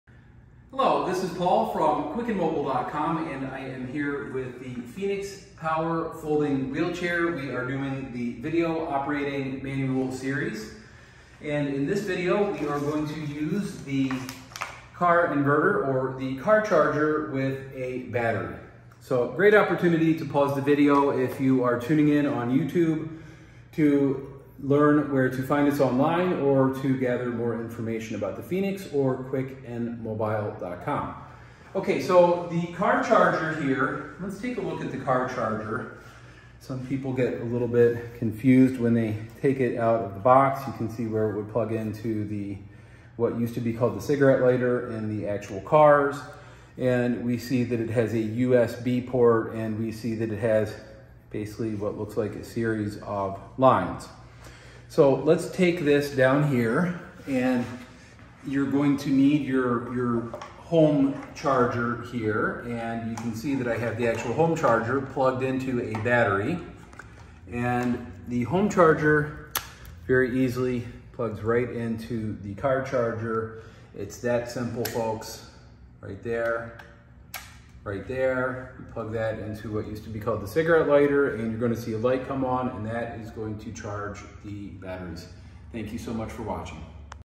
How to use the car charger with the Phoenix foldable power wheelchair
This is the video operating series demonstrating how to use the car charger with the Phoenix power folding wheelchair. For additional information, please visit